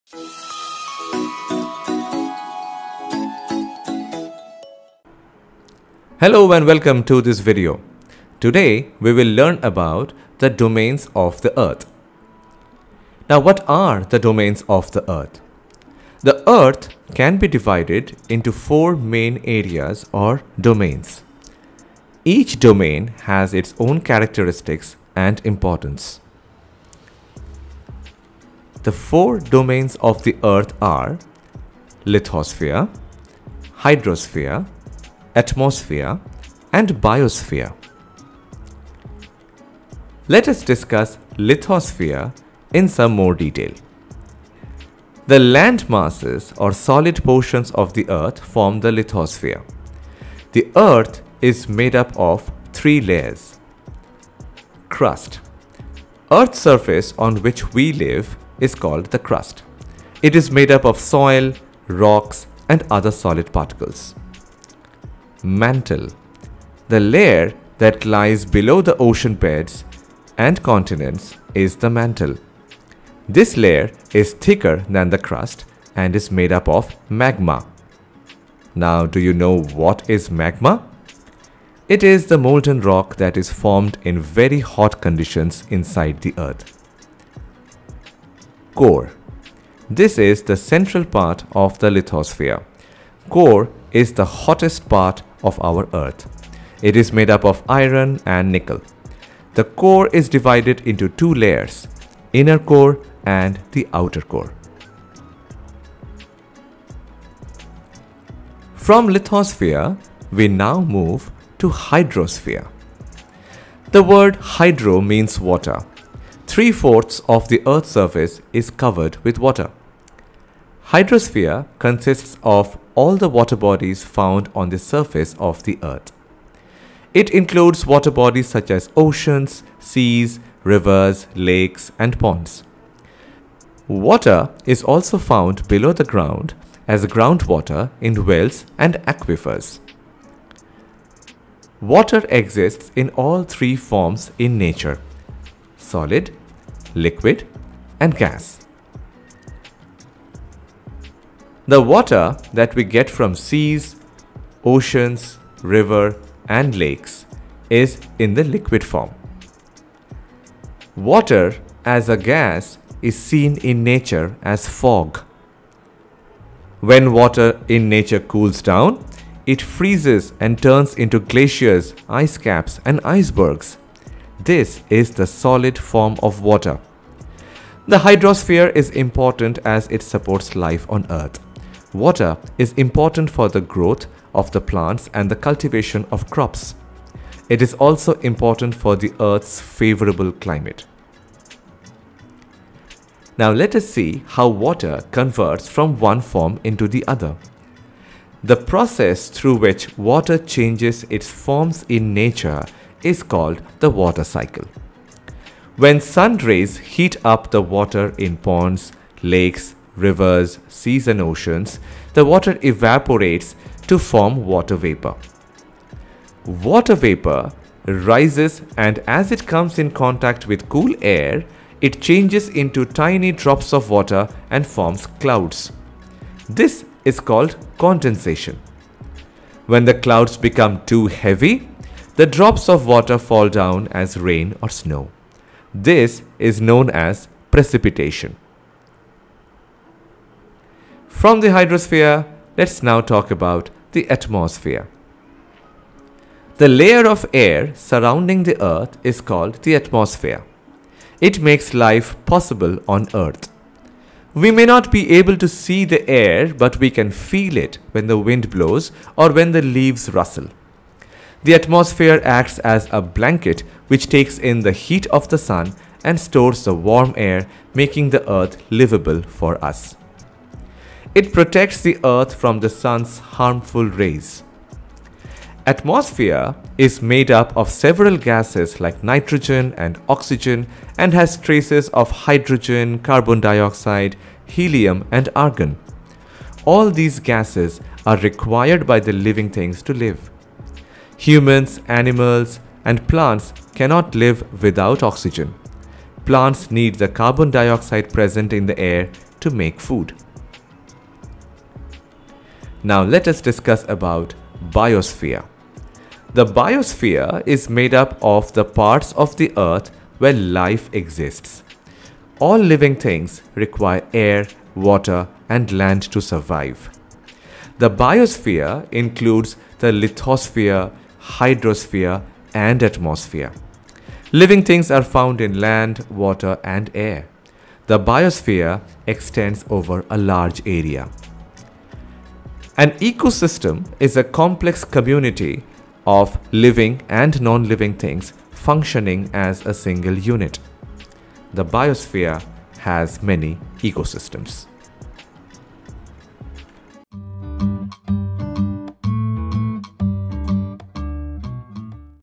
Domains of the Earth | Class 4 | ICSE | Social Studies SST | Quick Revision| NEP 2020 |CBSE
In this video we discuss the chapter on "Domains of the Earth".
Topics covered:
Domains of the Earth
Lithosphere
Biosphere
Atmosphere
Hydrosphere
Water Cycle
Forms of Water

This video is aimed to provide a quick revision before the exams.
ICSE sst Chapter 3 - As per NEP 2020 guidelines